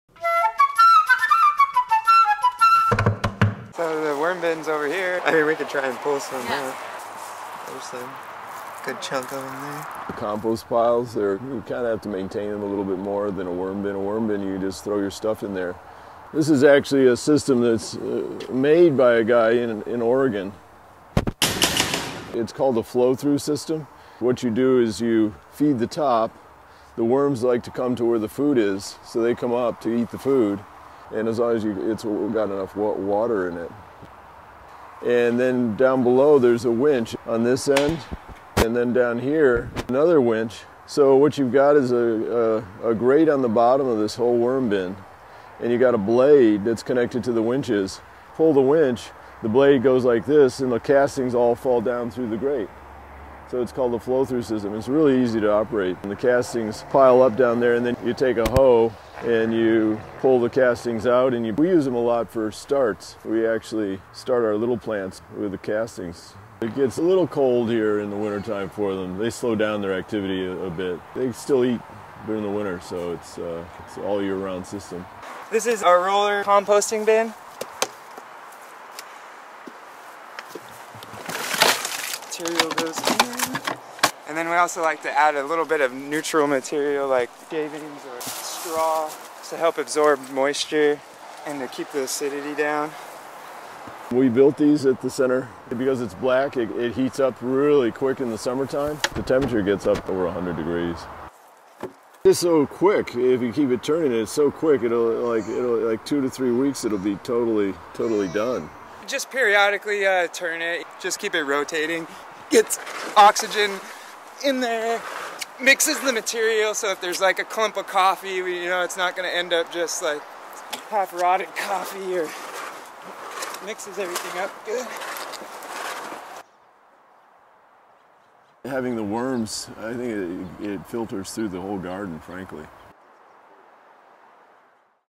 Rotary composter and flow-through worm bin for mass composting
In this video, Jan VanderTuin, of the Center for Appropriate Transport (CAT), showed us their flow through worm bin and a CAT invention- a rotary composter that heats up with the sun to make compost in just weeks.